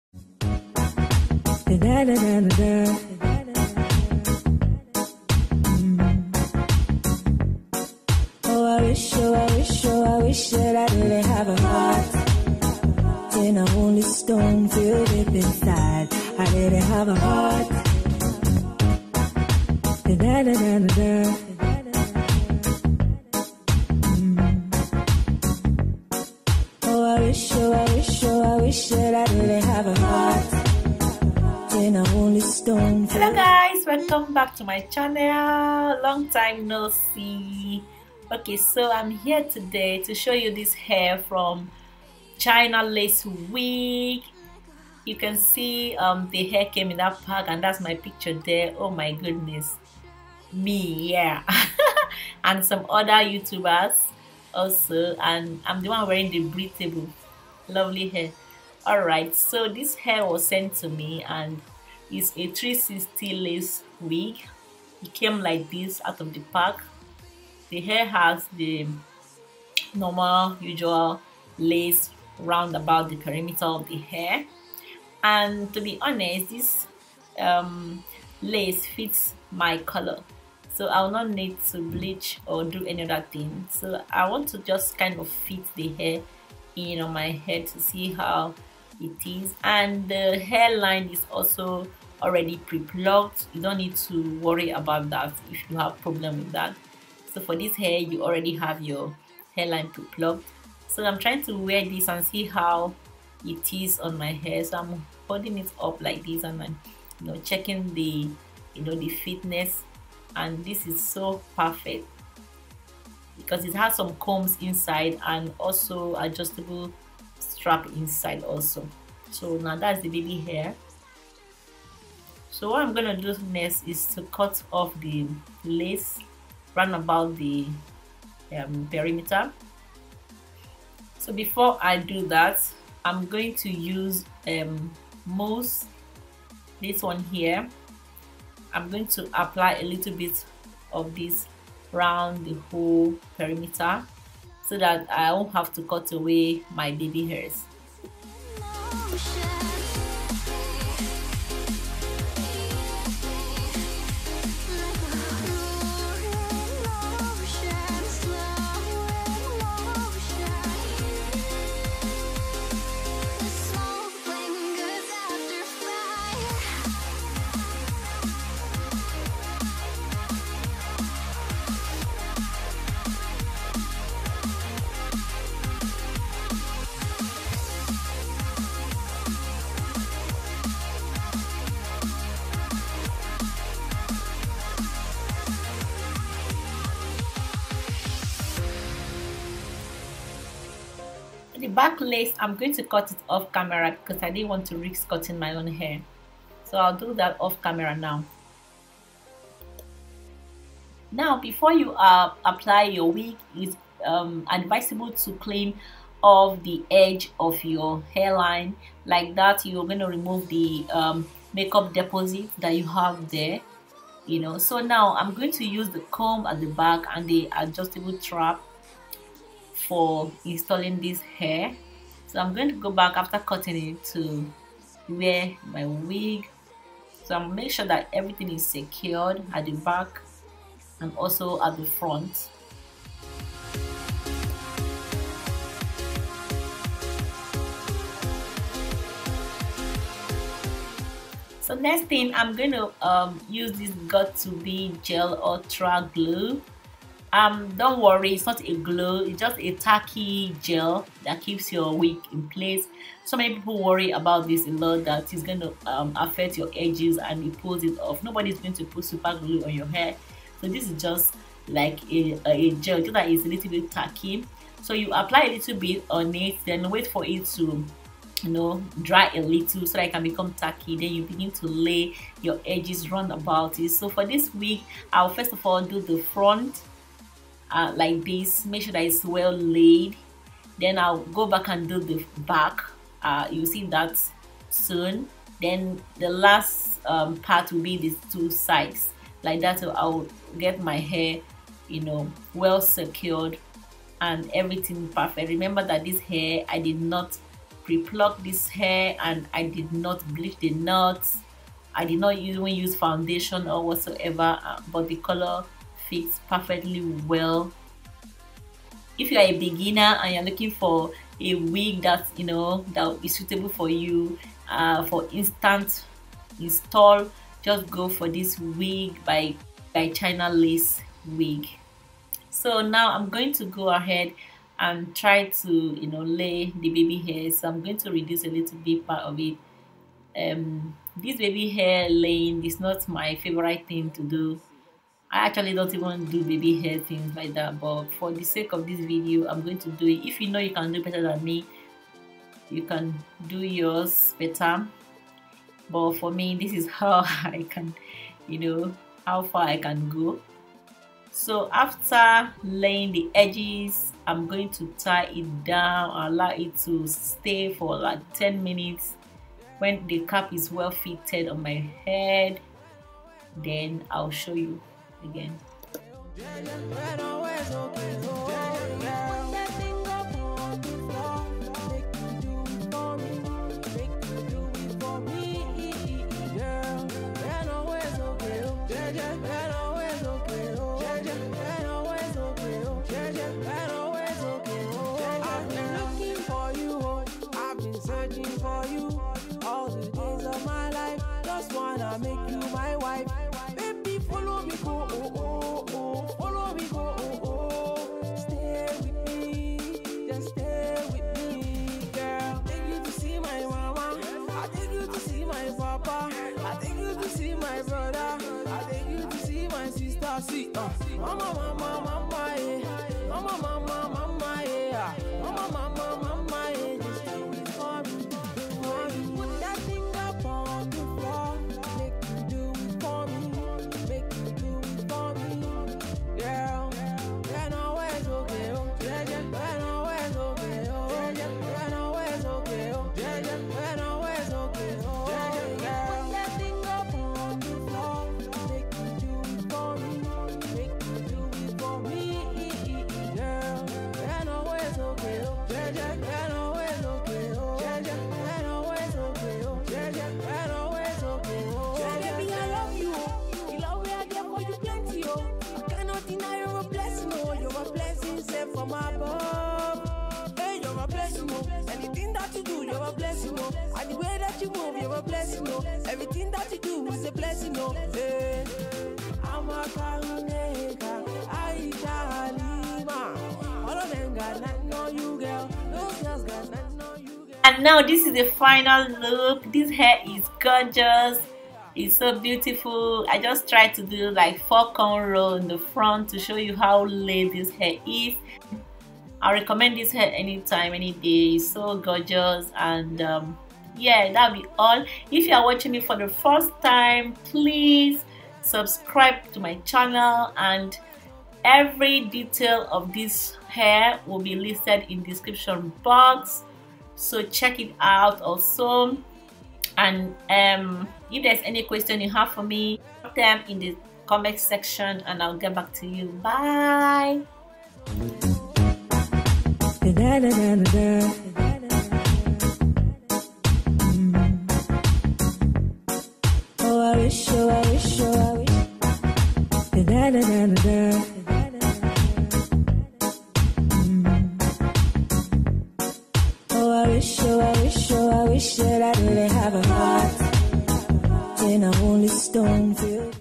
CHINALACEWIG INSTALL
READ INFO BELOW

360 lace wig
Hair Type: Virgin Brazilian Hair
Hair Style: Deep Wave
Hair length:22 inches
Hair Density:180%

360 Lace wig advantage:
1. Wig cap has strentch and comb, with natural hairline,

2. Can be ponytail

3. 4 inch deep part lace around your head

4. can extra hair density added

5. Big Elastic net,wear more comfortable

Recommended hair company

EMAIL FOR SERIOUS INQUIRY AND BUSINESS